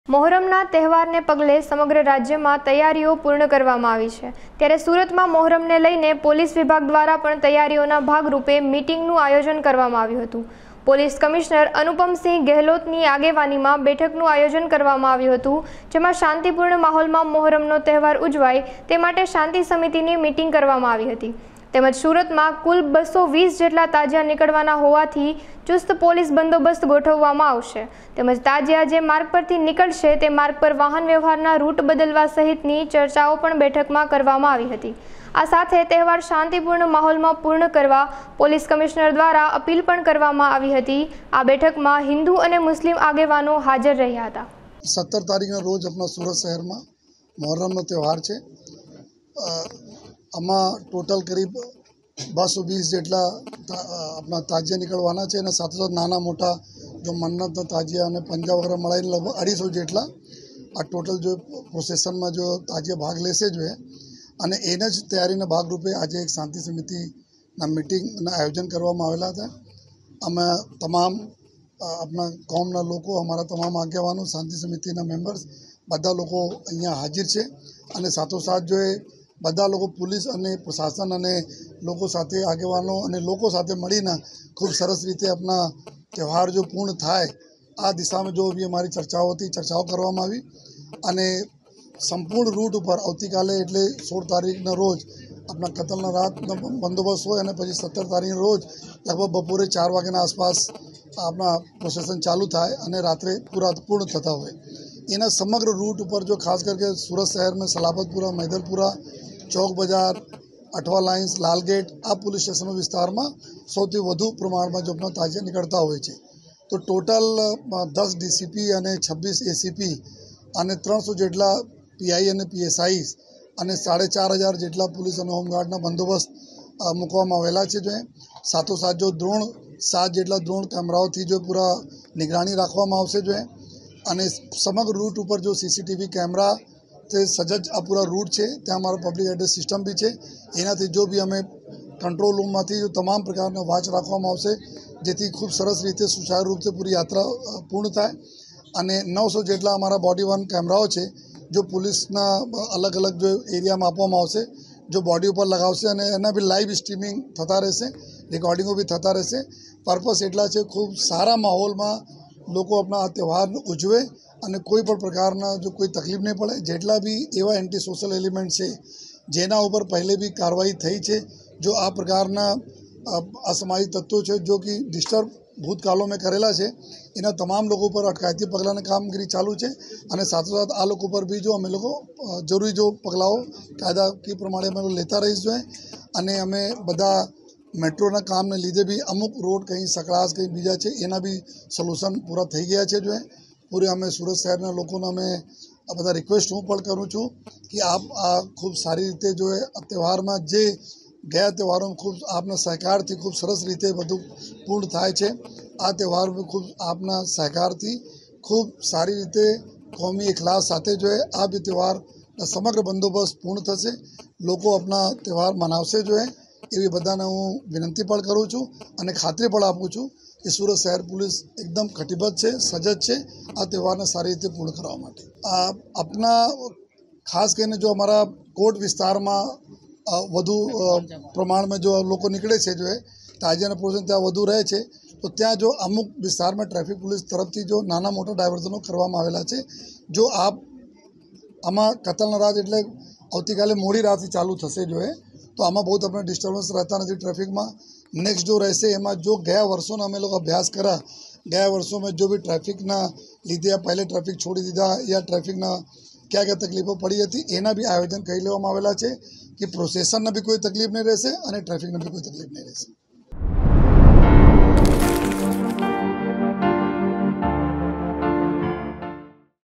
Surat : મોહરમના તહેવારને લઇ પોલીસ દ્વારા તૈયારીના ભાગ રૂપે બેઠક યોજાઈ
સુરત : મોહરમના તહેવારને લઇ પોલીસ દ્વારા તૈયારીના ભાગ રૂપે બેઠક યોજાઈ